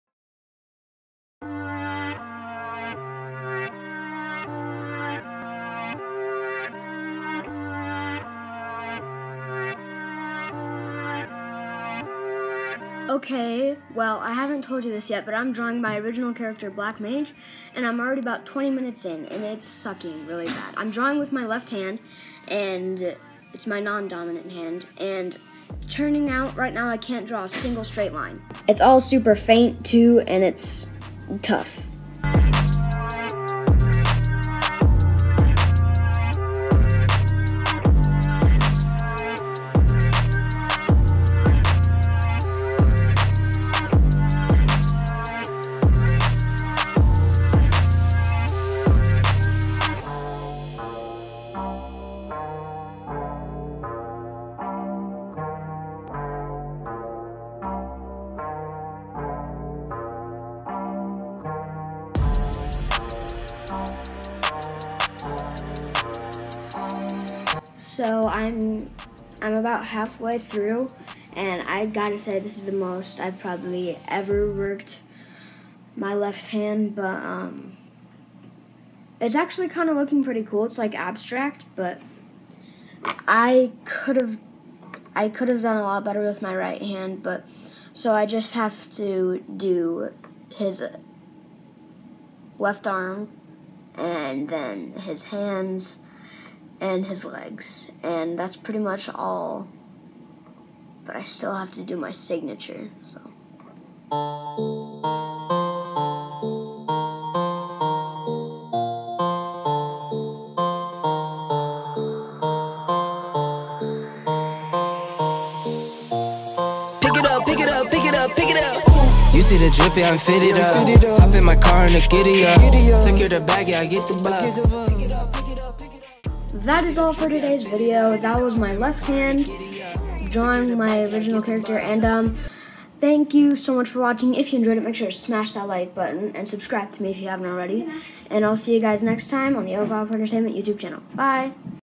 I tried to draw with my left hand!
So my original character black mage is being punished by being drawn by my left hand